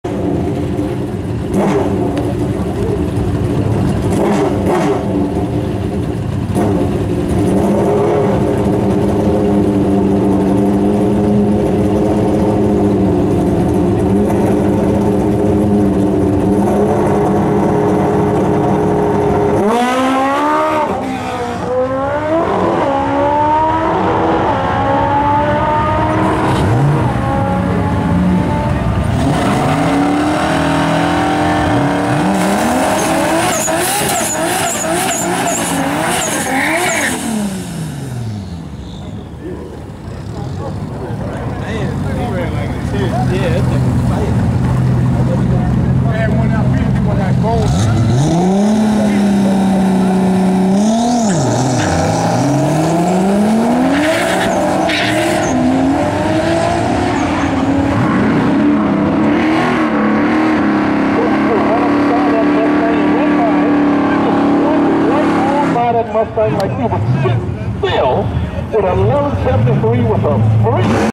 6th run - Outside view (Sony HD)
Turned high boost up even more than last few runs.  Start off in low boost (20psi). High boost activated from top of second gear on.  Bogged bad off the line. Started running lean second half of the track so so I let off 3/4 of the way through 4th.
11.733@135.24mph